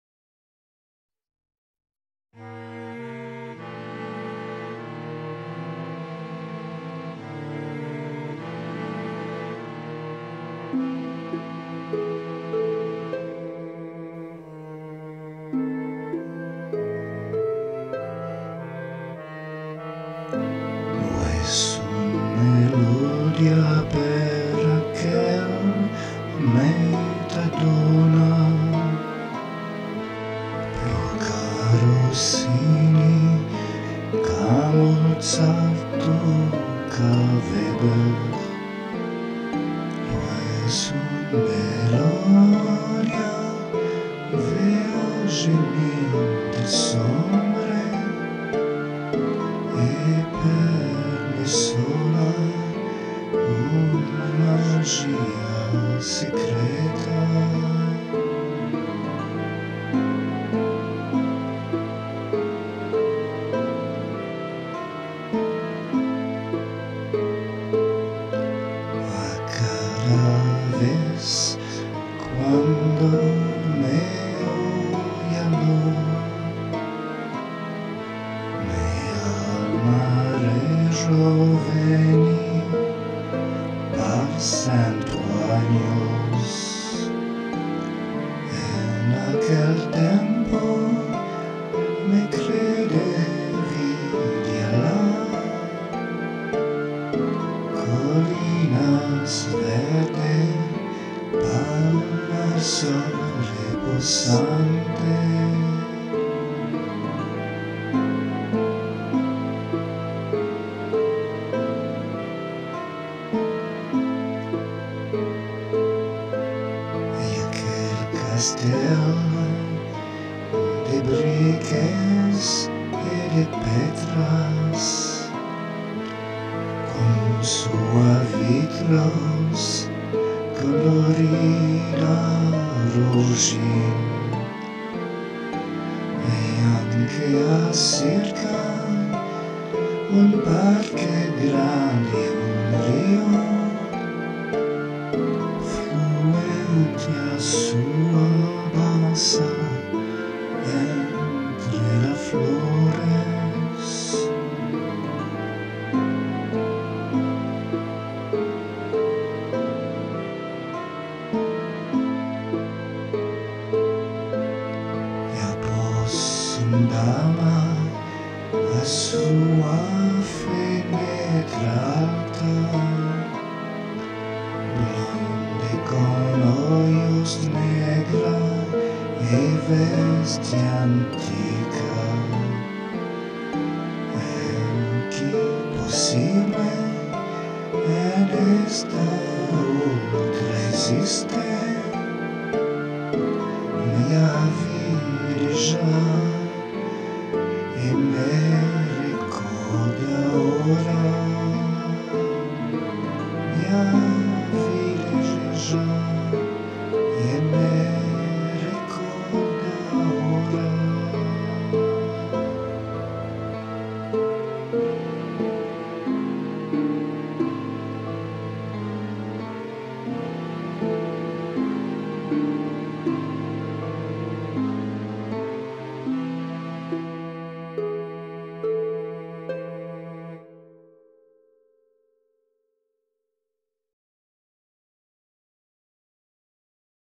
Magis Optis - La flor del deserto - Fantasia
Orijinal: _Fantaisie_ , Gérard de Nerval (1852). Depinta de Yves Gaillard: _La Flor del deserto_ (1972).

Fantasia

Lo es un melodia, per acel me ta dona
Plu ca Rossini, ca Mozart, o ca Weber,
Lo es un melodia, vea, jeminte, sombre,
E, per me sola, un majia secreta.

A cada ves, cuando me oia lo,
Mea alma rejoveni par sento anios:
En acel tempo, me crede vide ala
Colinas verde, pal al sol reposante,

E acel castel, de brices e de petras,
Con sua vitros colorida rojin,
E, ance a sirca, un parce grande, e un rio,
Fluente a sua basa, entre la flores.

A pos, un dama, a sua fenetra alta,
Blonde con oios negra, e veste antica:
El ci, posible, en esta otra esiste,
Me ia vide ja, e me recorda aora!